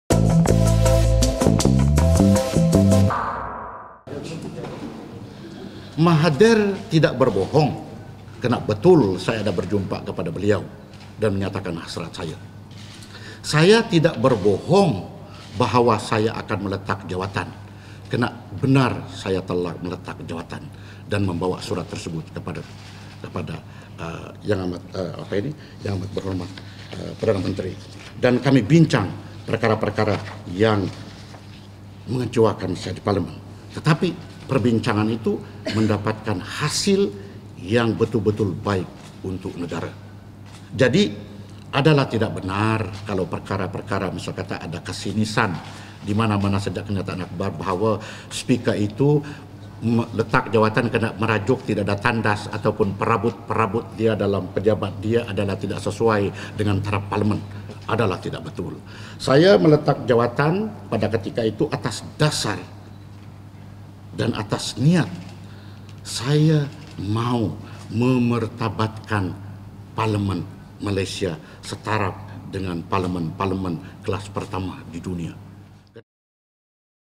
Pandikar: Nobody lied but I did resign!
Ini Kali Lah was Dewan Rakyat Speaker Tan Sri Pandikar Amin Mulia's battle cry to transform the Parliament. Since his demands were considered, he has rescinded his March 18th resignation letter.